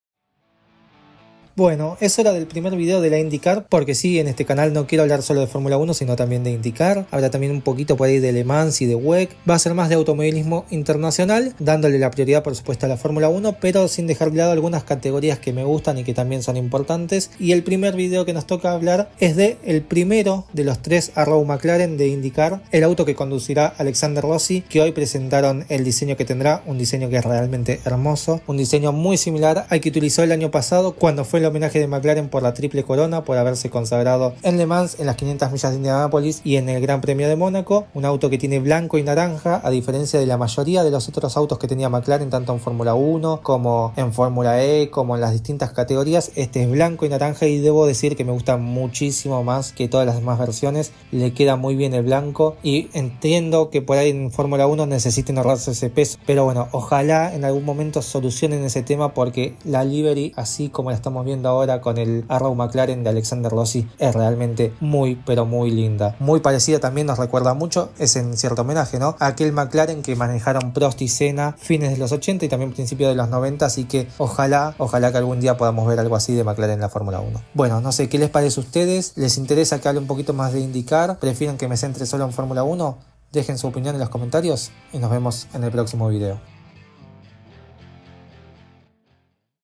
Primeras Impresiones: ¡ASÍ debería ser el MCLAREN de F1! | INDYCAR
Llegó el primer video de la Indycar, dónde comento un poco la espectacular livery del Arrow McLaren de Alexander Rossi.

¡Ojalá McLaren se anime a algo así de manera permanente, pronto!